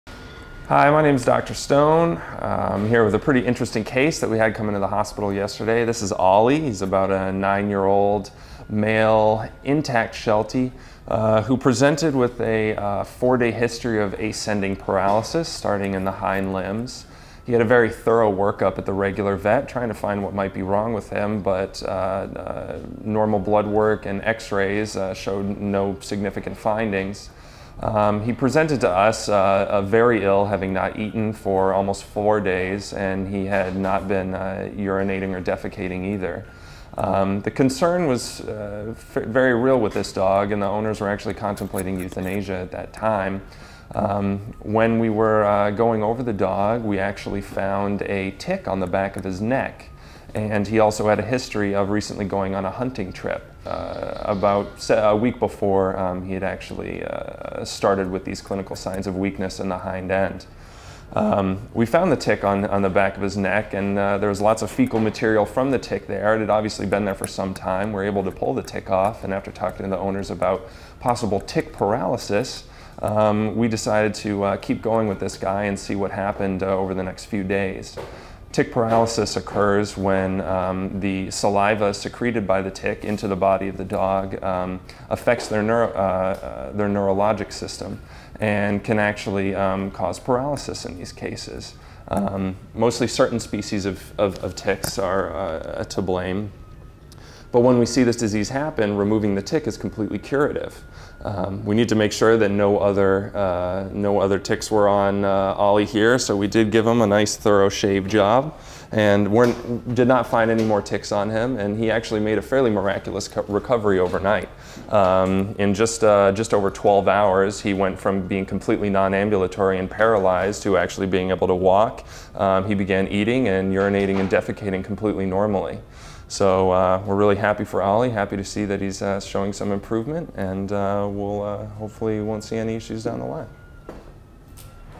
Tick Paralysis
An overview of a case of tick paralysis or tick bite paralysis and the amazing recovery made once the tick was removed from the patient. Tick poisoning, or tick paralysis, is a serious and potentially fatal condition requiring urgent veterinary attention. Ticks attach to the pet and a toxin in the tick's saliva attacks their nervous system.